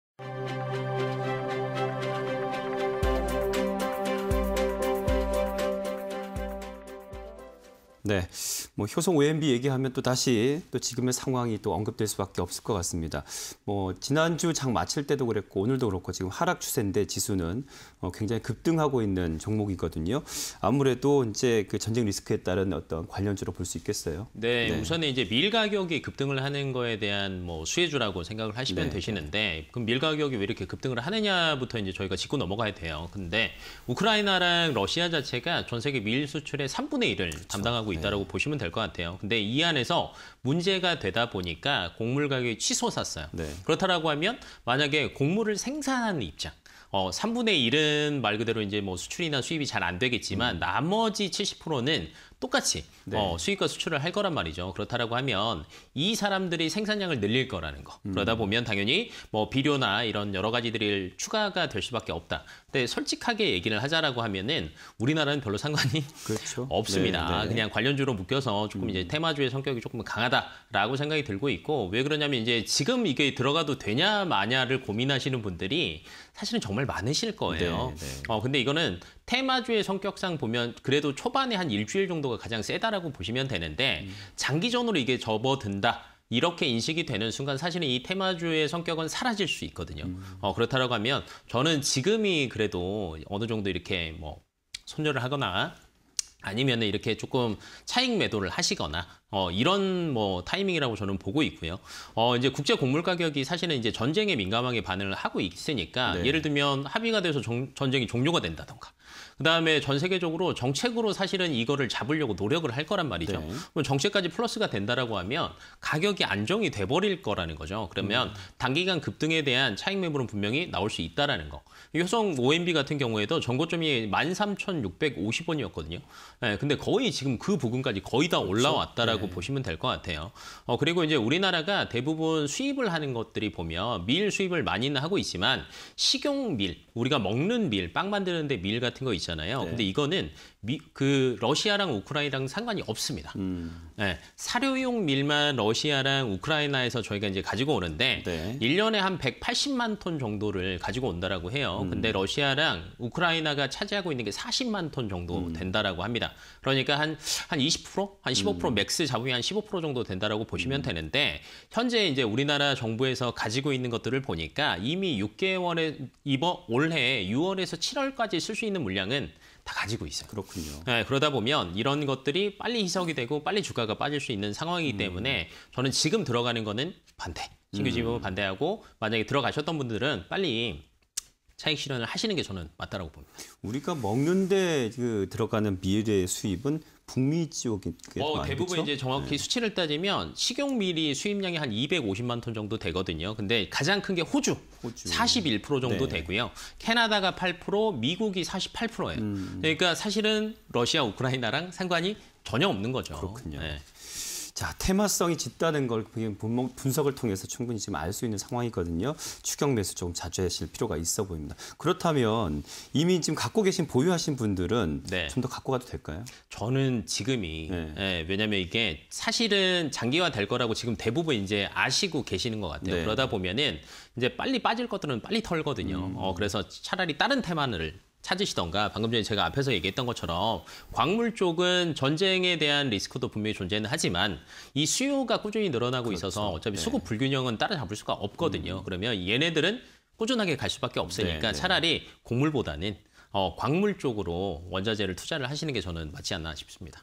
효성오앤비(097870)... 국제 밀 가격 14년 만에 최고치_테마IN이슈 (20220307)
효성오앤비(097870)
- 국제 밀 가격 14년 만에 최고치
- 치솟는 곡물값에 국내 비료업체 강세
- 우크라이나 및 러시아, 밀 수출 약 29% 담당
- 가격 상승에 농산물 공급 확대 움직임… 비료 수요 증가